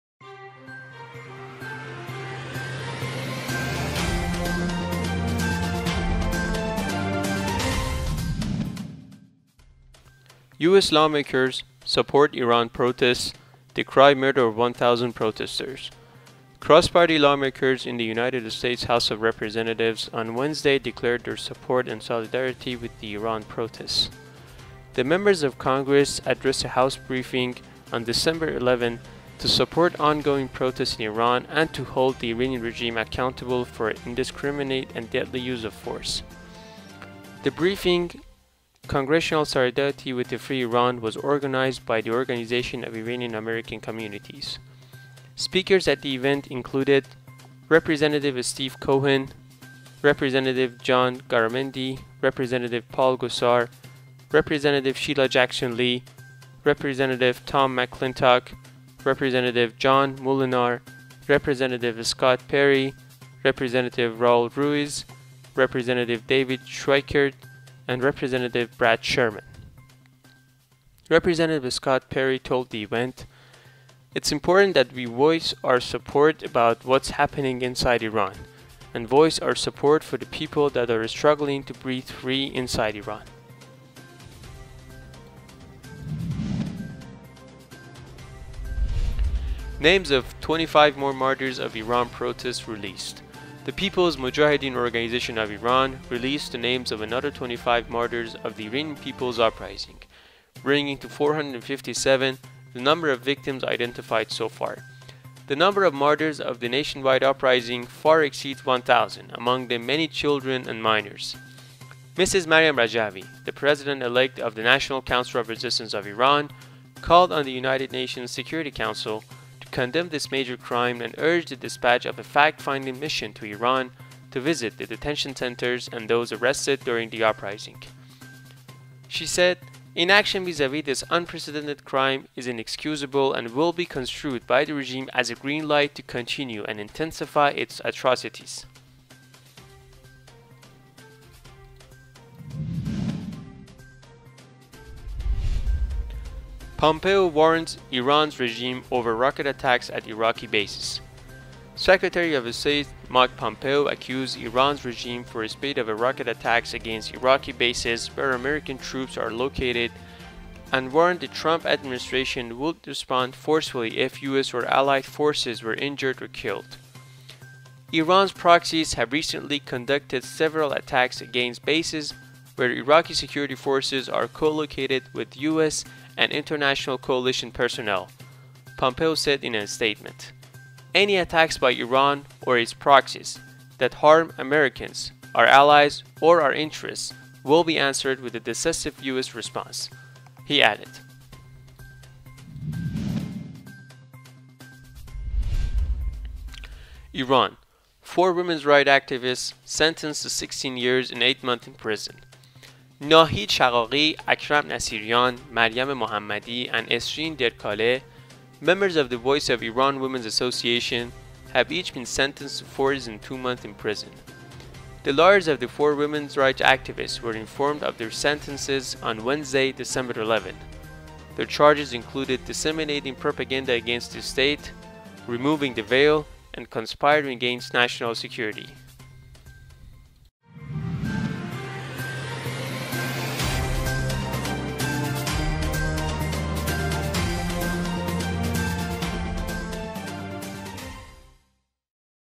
Iran news in brief, December 14, 2019
U.S. Lawmakers Support Iran Protests, Decry Murder of 1000 Protesters

Cross-party lawmakers in the United States House of Representatives on Wednesday declared their support and solidarity with the Iran protests.

The Members of Congress addressed a House briefing on December 11, to support ongoing protests in Iran and to hold the Iranian regime accountable for indiscriminate and deadly use of force.

The briefing “Congressional Solidarity with a Free Iran” was organized by the Organization of Iranian American Communities.

Speakers at the event included: Rep. Steve Cohen, Rep. John Garamendi, Rep. Paul Gosar, Rep. Sheila Jackson Lee, Rep. Tom McClintock, Rep. John Moolenaar , Rep. Scott Perry, Rep. Raul Ruiz, Rep. David Schweikert , and Rep. Brad Sherman .

Rep. Scott Perry told the event: "It's important that we voice our concerns about what's happening inside Iran. And voice our support for the people that are struggling to breathe free inside of Iran.

Names of 25 More Martyrs of Iran Protests Released

The People’s Mojahedin Organization of Iran released the names of another 25 martyrs of the Iranian people’s uprising, bringing to 457 the number of victims identified so far.  The number of martyrs of the nationwide uprising far exceeds 1,000, among them many children and minors.

Mrs. Maryam Rajavi, the President-elect of the National Council of Resistance of Iran called on the United Nations Security Council to condemn this major crime and urged the dispatch of a fact-finding mission to Iran to visit the detention centers and those arrested during the uprising.  Inaction vis-à-vis this unprecedented crime is inexcusable and will be construed by the regime as a green light to continue and intensify its atrocities, she said.

Pompeo Warns Iran's Regime Over Rocket Attacks at Iraqi Bases

Secretary of State Mike Pompeo accused Iran’s regime for a spate of rocket attacks against Iraqi bases where American troops are located and warned the Trump administration would respond forcefully if U.S. or allied forces were injured or killed.

“Iran’s proxies have recently conducted several attacks against bases where Iraqi Security Forces are co-located with U.S. and International Coalition personnel,” Mr. Pompeo said in a statement.

Any attacks by Iran or its proxies “that harm Americans, our allies or our interests will be answered with a decisive U.S. response,” Pompeo added.

Iran: Four Women’s Rights Activists Sentenced to 16 Years and 8 Months in Prison

Nahid Shaqaqi, Akram Nasirian, Maryam Mohammadi, and Esrin Derkaleh, members of the Voice of Iran Women’s Association have each been sentenced to 4 years and 2 months in prison. The lawyers of the four women’s rights activists were informed of their sentences on Wednesday, December 11, Their charges included disseminating propaganda against the state, removing the veil, and conspiring against national security.